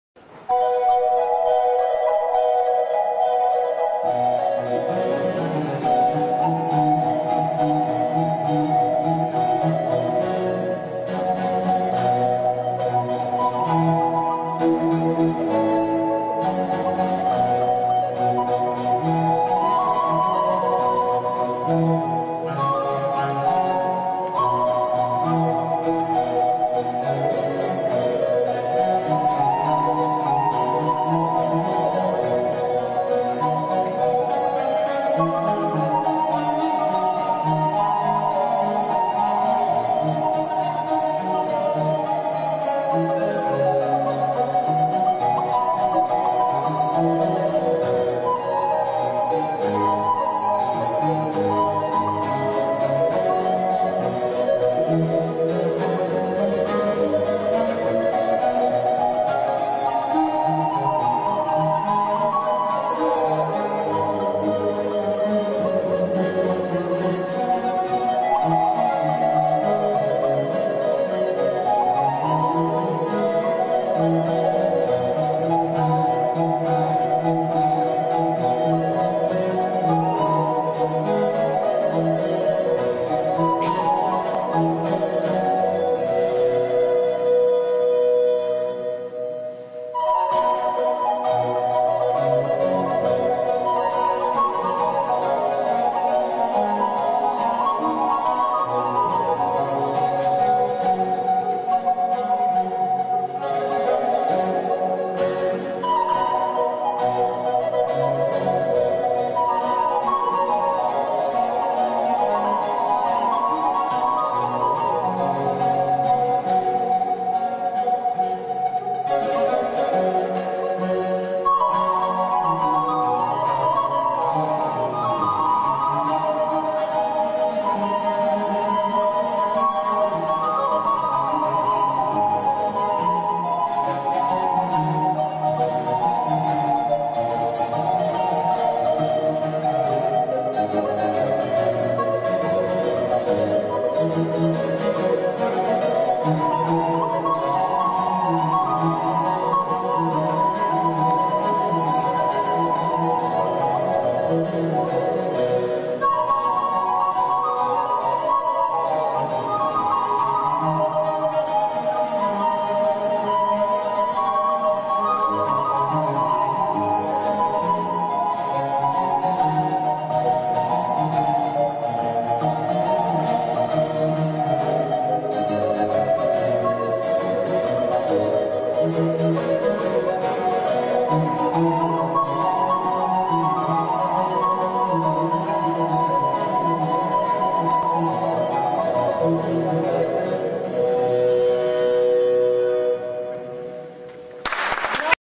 bouncing ball jc schickhardt
die musik ist von johann christian schickhardt weil die rechenmaschine  von wilhelm schickhardt entwickelt wurde.